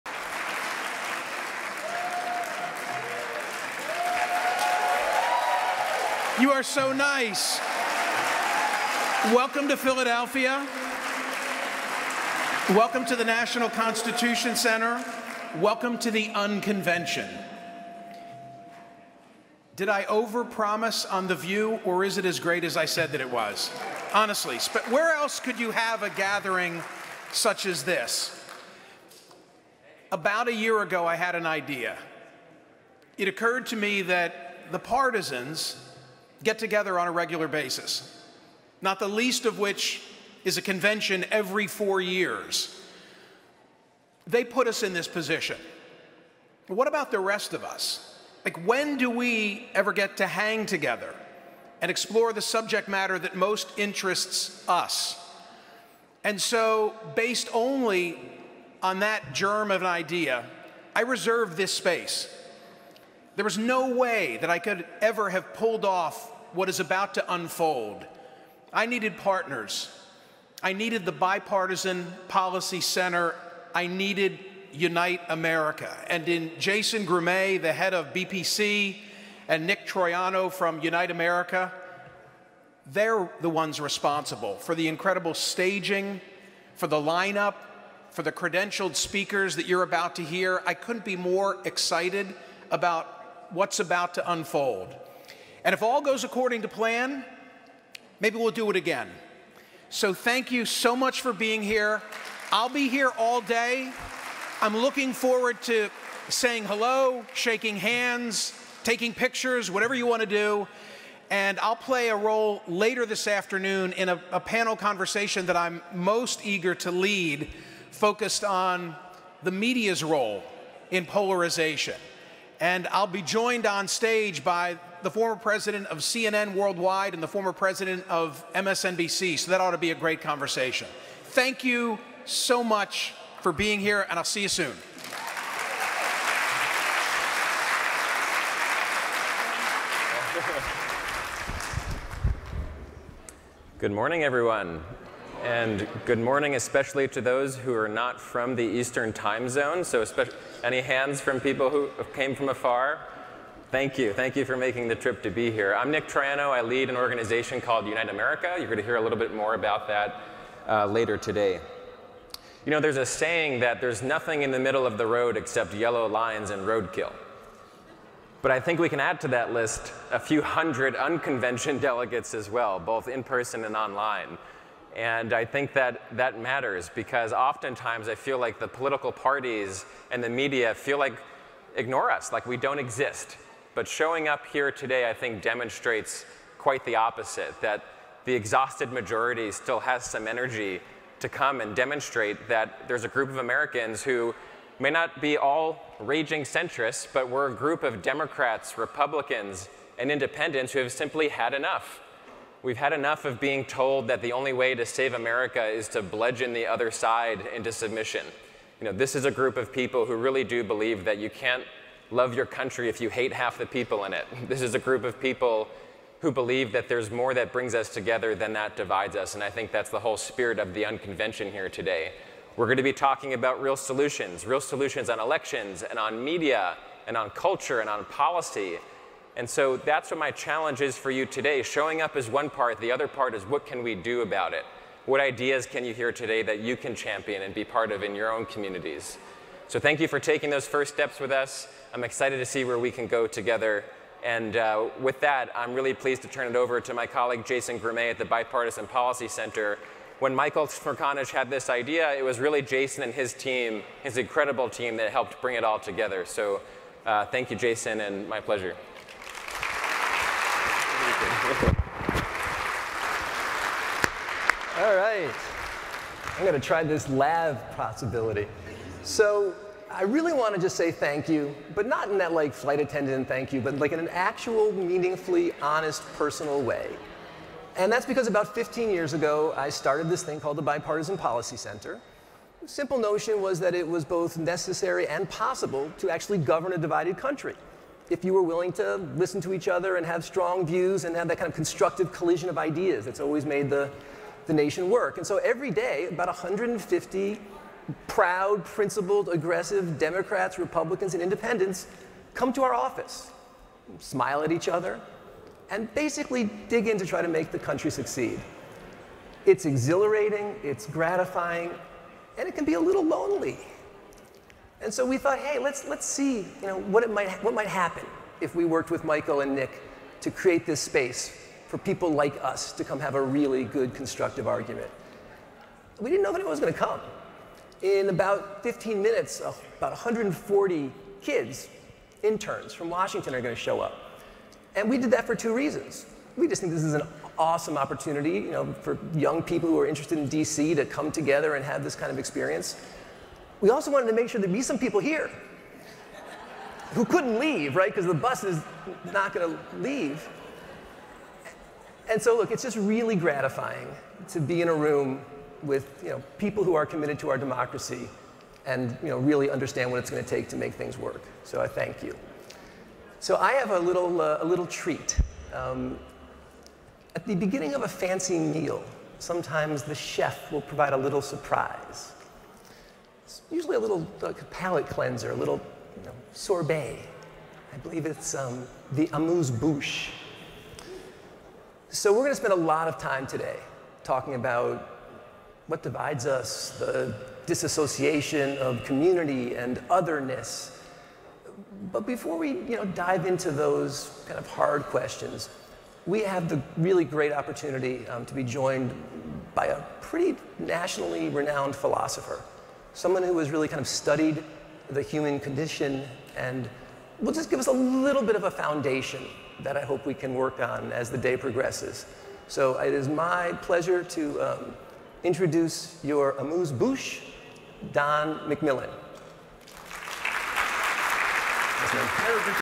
Un-Convention Welcome
Attendees are welcomed by representatives from Bipartisan Policy Center, Smerconish, and Unite America:
Jason Grumet, BPC
Michael Smerconish, Smerconish.com; SiriusXM; CNN
Nick Troiano, Unite America